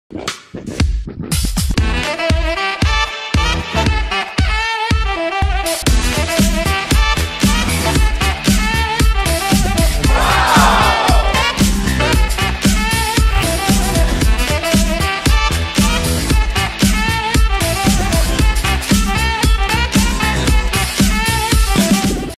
남자들을 위한 신개념 면도날 전용 전동크리너 '레이저부스터'
면도'날' 바꾸지 말고 새것처럼 강력세척!